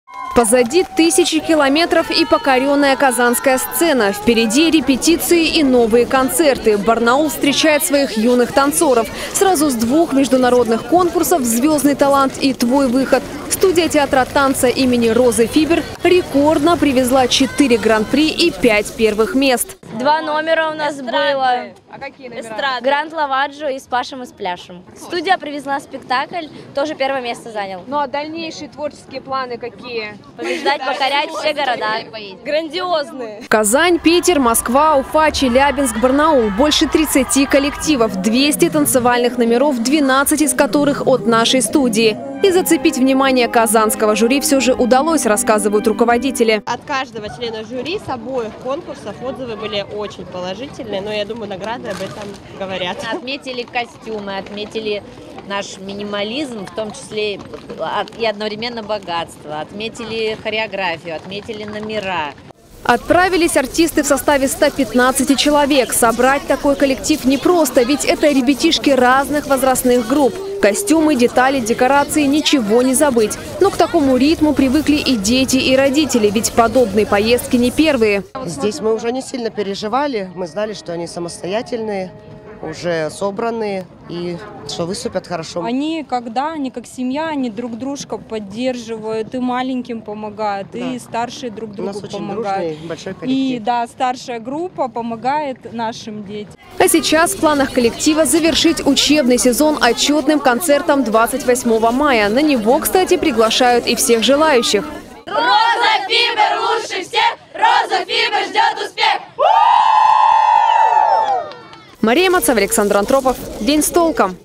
Студия театра танца имени Розы Фибер одержала победу в международных конкурсах
Четырежды Лауреаты первой степени, обладатели Гран-При – с таким победным кушем из Казани вернулся коллектив студии театра танца имени Розы Фибер. Дома победителей встречали с цветами и шариками.